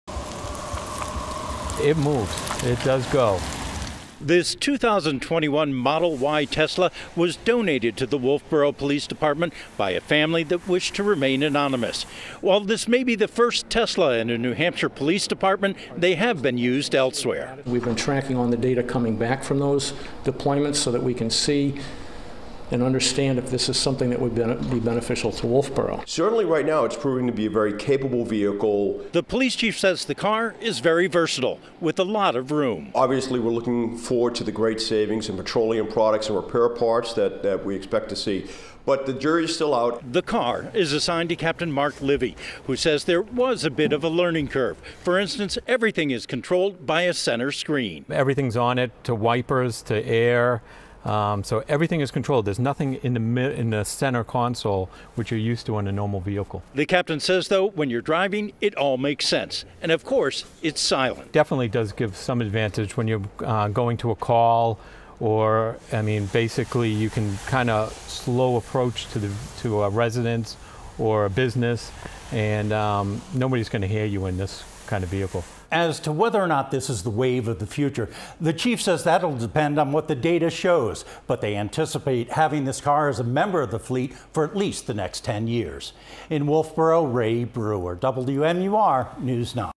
Wolfeboro police test Tesla cruiser
The electric vehicle was donated to the department by a family who wished to remain anonymous.